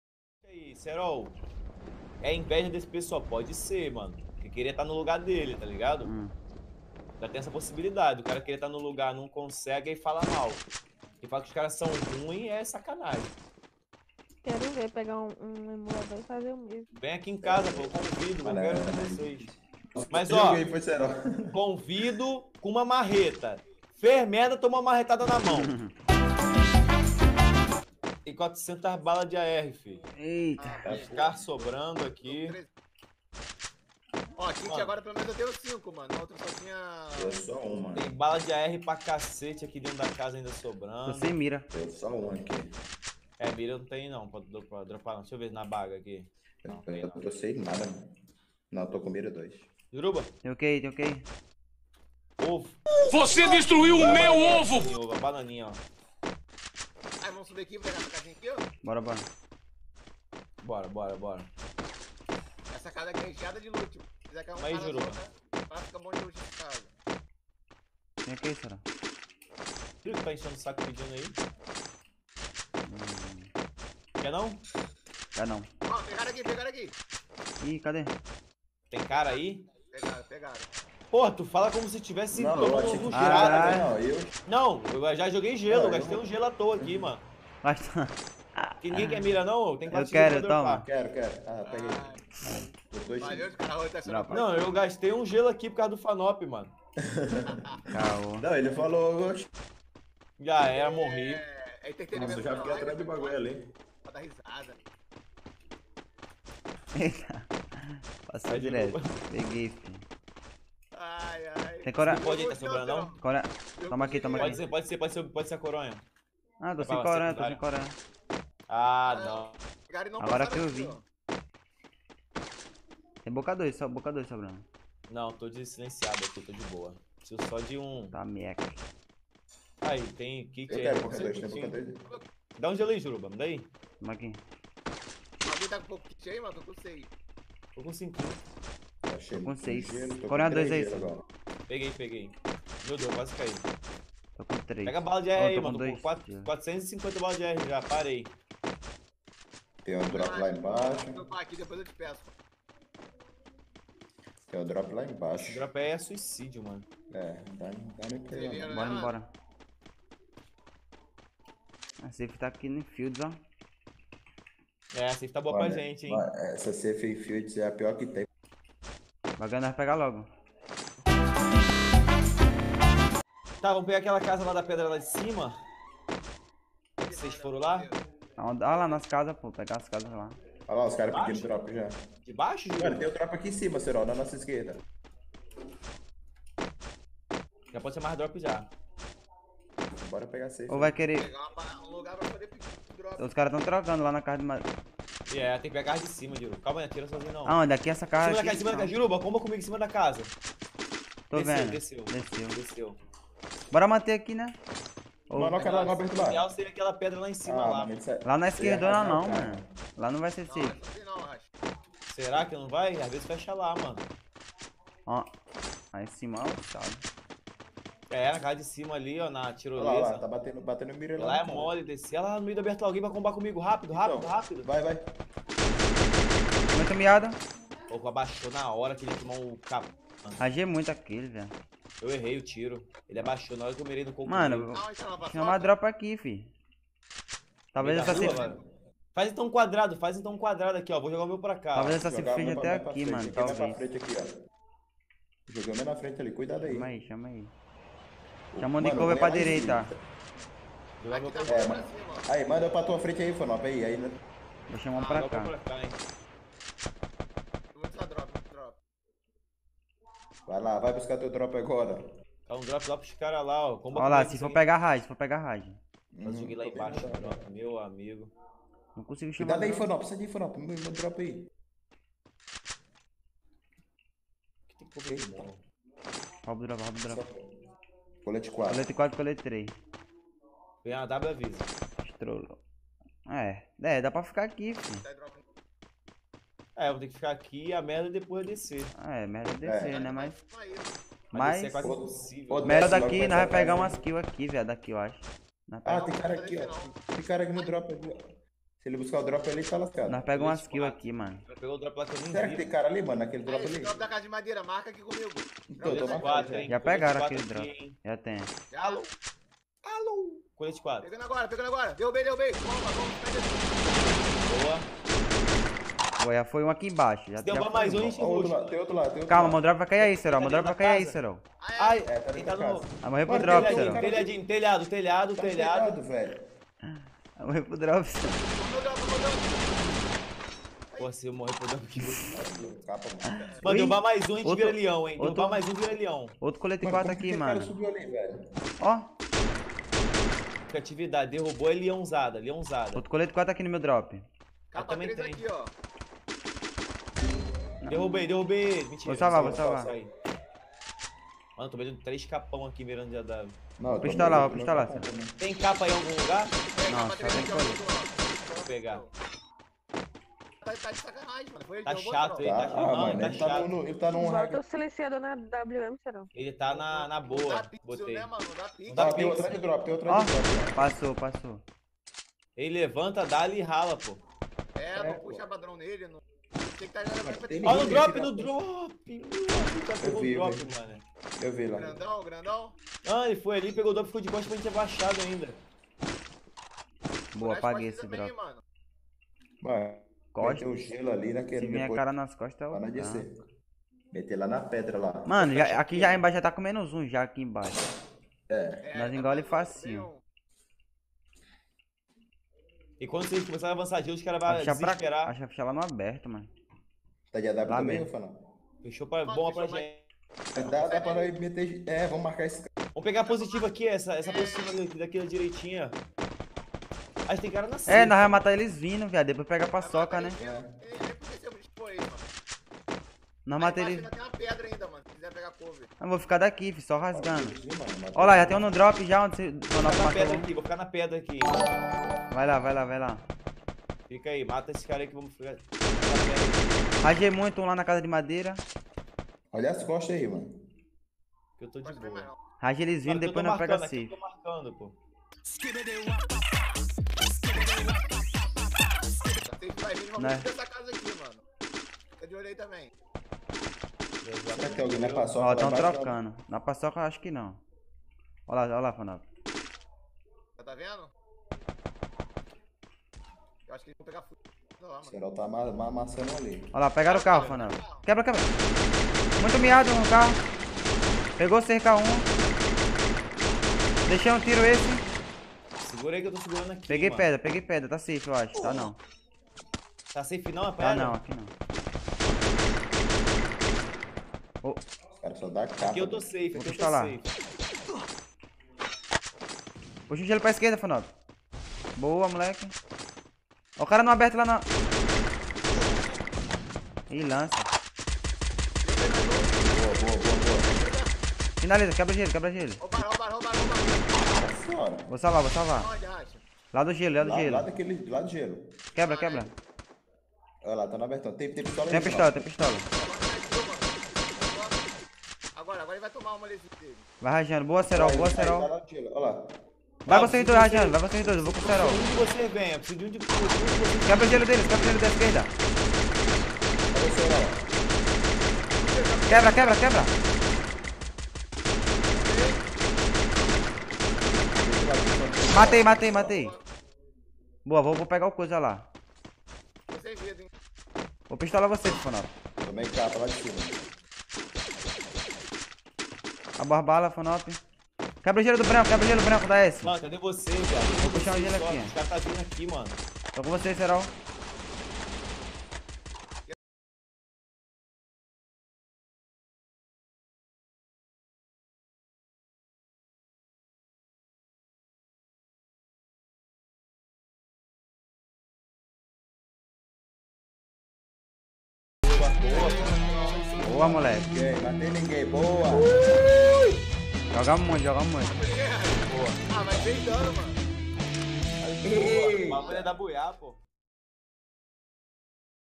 CEROL SE IMPRESSIONOU COM ESSA JOGADA!! AMASSAMOS NO XTREINO ft CEROL, EL RACHA E FANNOP
Deixa o likezão quem gostou do video e compartilha pra geralll TMJ É NOIS

Rapaziada, não consegui botar a foto do fannop na thumb, pq não achei nenhuma foto dele!! mas é isso fannop é brabo